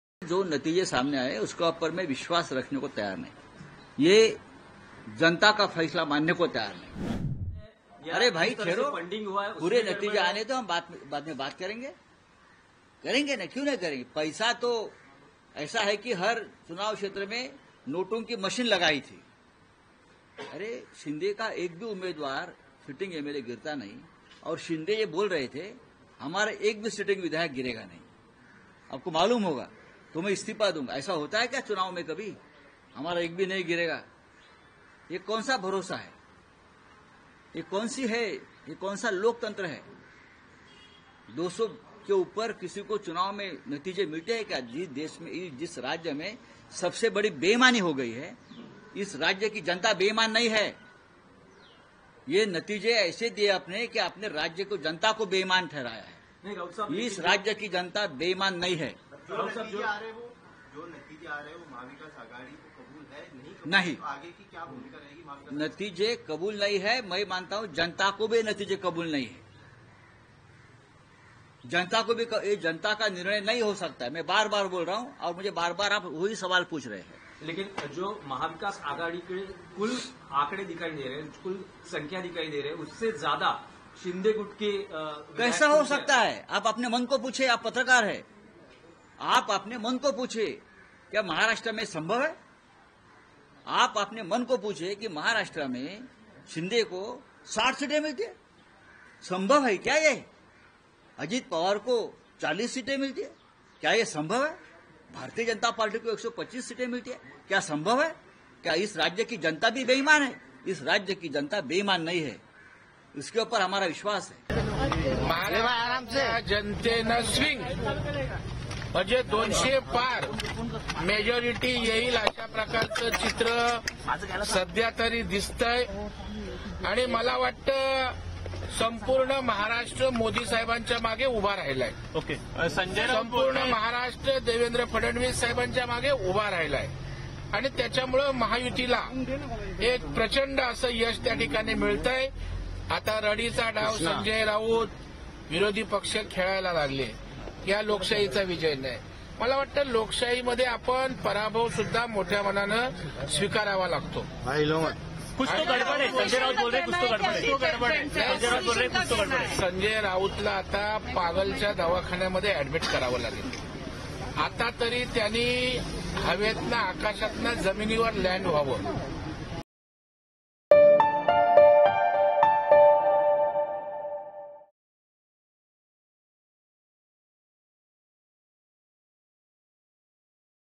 Maharashtra Election: Shiv Sena UBT Claims 'Gadbad With Note Machine'; BJP's 'Mental Hospital' Reply
As the MVA faces a major defeat in Maharashtra, Shiv Sena (UBT) leader Sanjay Raut has rejected the results, claiming they do not reflect the will of the people. He accused the Mahayuti of manipulating the system, questioning, 'How can all of Shinde’s candidates win?' and emphasized, 'The people of this state are not dishonest.